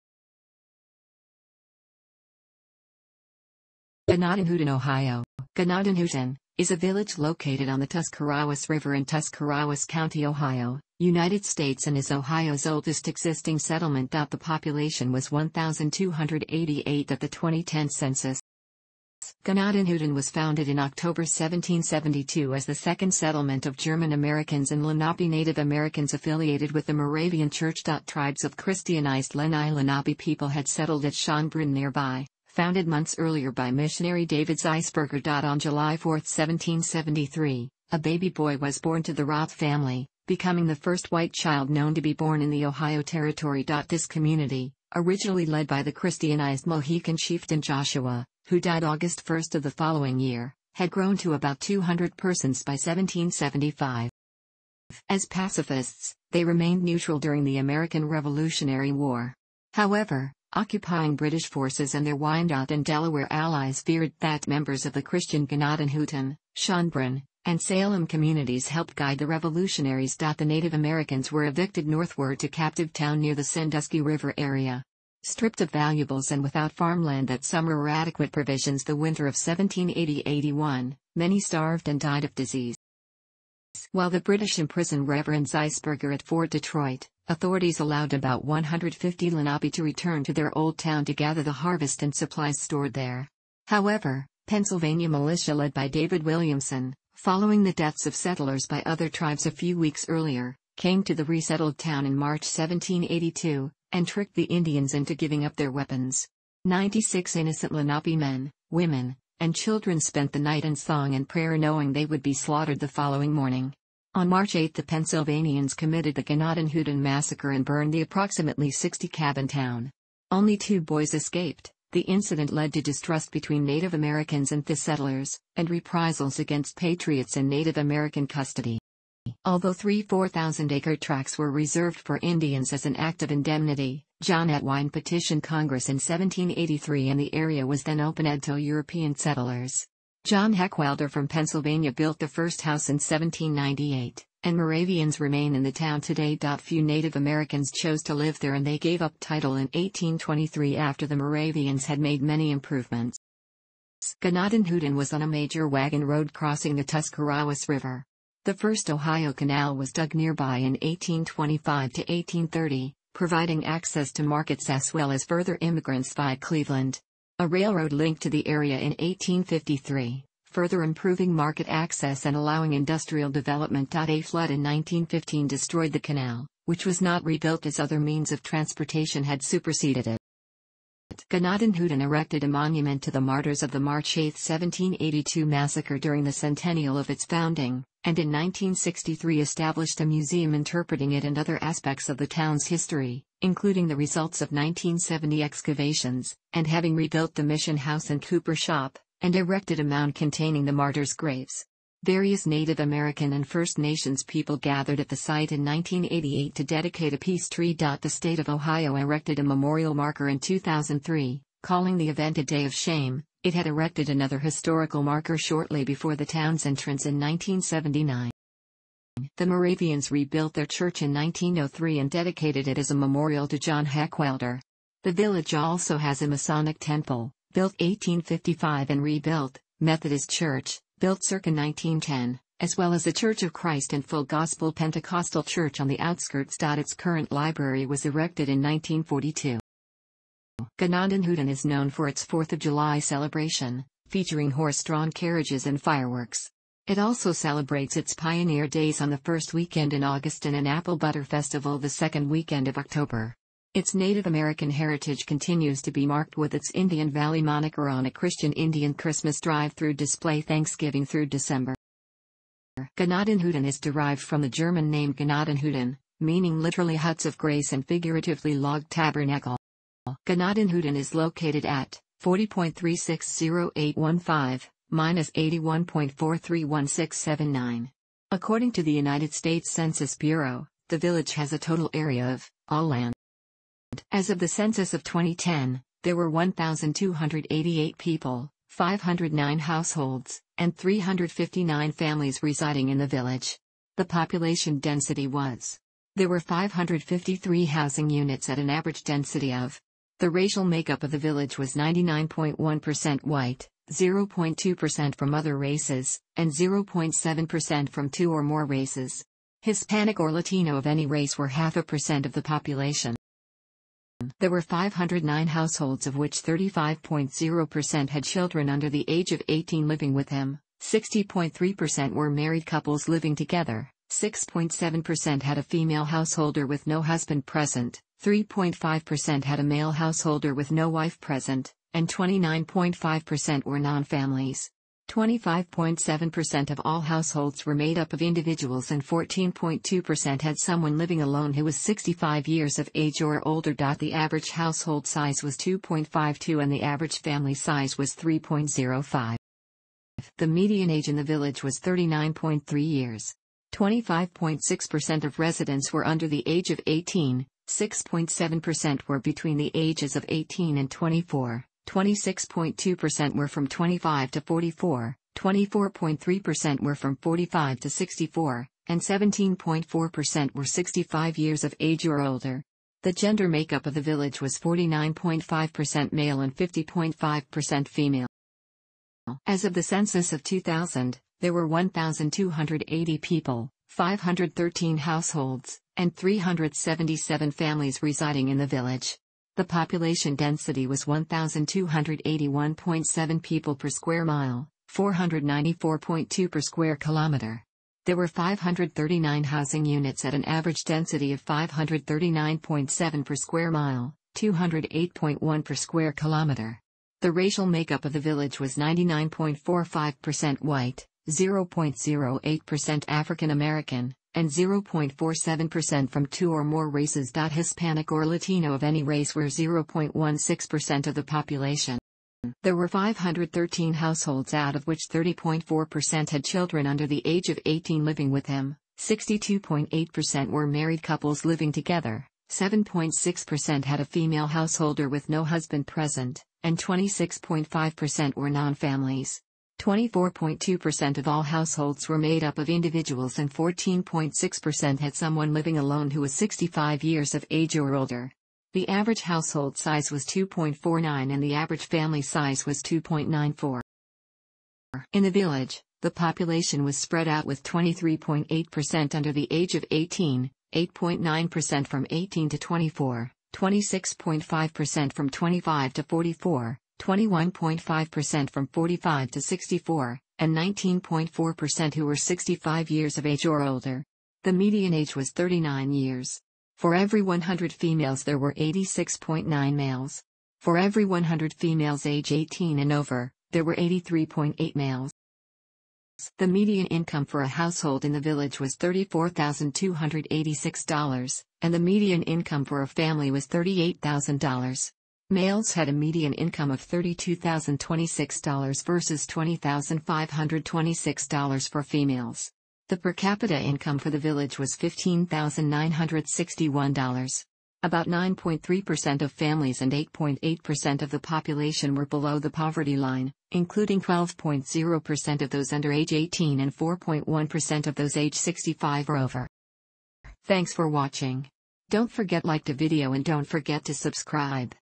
Gnadenhutten, Ohio
Gnadenhutten ( ) is a village located on the Tuscarawas River in Tuscarawas County, Ohio, United States and is Ohio's oldest existing settlement. The population was 1,288 at the 2010 census.

Tribes of Christianized Lenni Lenape people had settled at Schoenbrunn nearby, founded months earlier by missionary David Zeisberger. On July 4, 1773, a baby boy was born to the Roth family, becoming the first white child known to be born in the Ohio territory. This community, originally led by the Christianized Mohican chieftain Joshua (who died August 1 of the following year), had grown to about 200 persons by 1775.

As pacifists, they remained neutral during the American Revolutionary War. However, occupying British forces and their Wyandot and Delaware allies feared that members of the Christian Gnadenhutten, Schoenbrunn, and Salem communities helped guide the revolutionaries. The Native Americans were evicted northward to "Captive Town" near the Sandusky River area. Stripped of valuables and without farmland that summer or adequate provisions the winter of 1780-81, many starved and died of disease.

While the British imprisoned Rev. Zeisberger at Fort Detroit, authorities allowed about 150 Lenape to return to their old town to gather the harvest and supplies stored there. However, Pennsylvania militia led by David Williamson, following the deaths of settlers by other tribes a few weeks earlier, came to the resettled town in March 1782, and tricked the Indians into giving up their weapons. Ninety-six innocent Lenape men, women, and children spent the night in song and prayer knowing they would be slaughtered the following morning. On March 8 the Pennsylvanians committed the Gnadenhutten massacre and burned the appro...